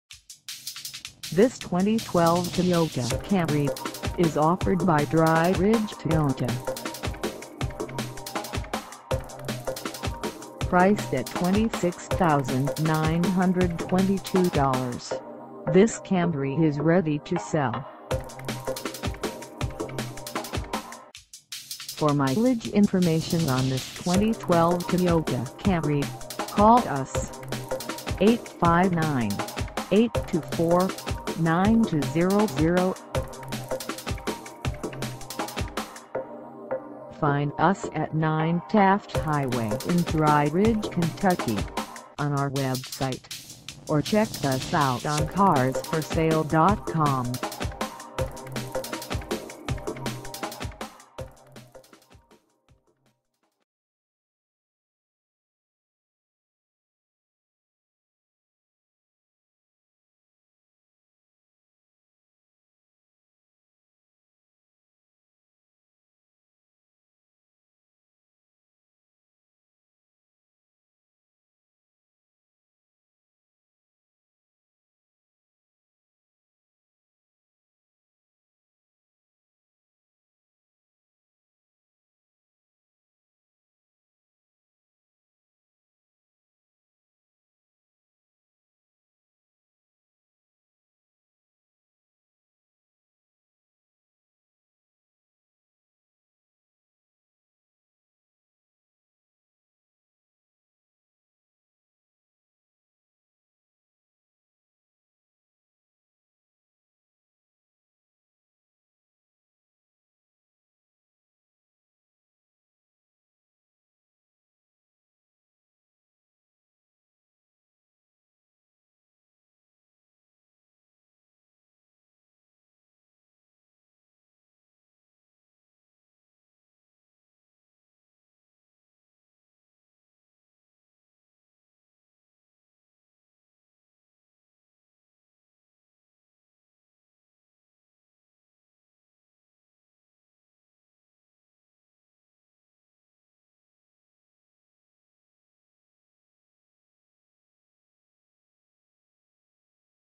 2012 Toyota Camry SE - for sale in Dry Ridge, KY 41035
9 Taft Highway

Come test dirve this 2012 Toyota Camry SE for sale in Dry Ridge, KY.

 S